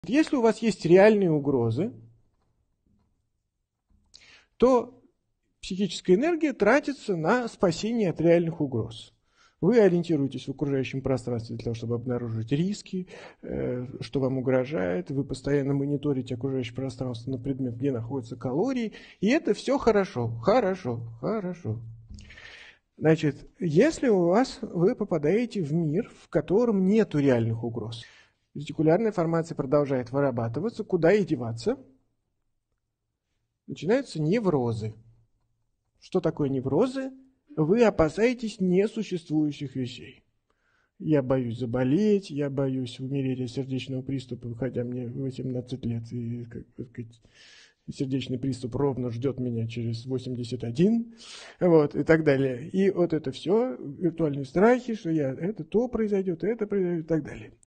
Откуда родом ваши неврозы | Андрей Курпатов
Мы сами создаём наши неврозы, потакая собственному инстинкту самосохранения.

Так, в отсутствие реальных угроз мы усердно защищаем себя от вымышленных.

Смотрите видео Андрея Курпатова, чтобы понять, откуда родом ваши неврозы 👆🏼

❓А как вы думаете, ваши собственные тревоги основаны на «реальных событиях»?
Или вы просто нашли, чем занять свой инстинкт самосохранения? Делитесь в комментариях.